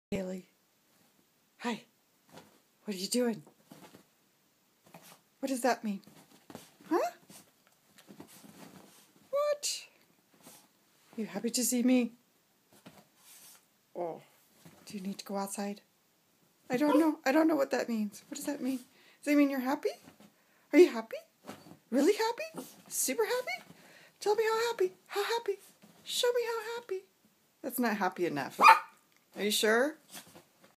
Happy Hailey
Hailey, the Malti-Silky-Poo-HuaHua tells me she's happy today!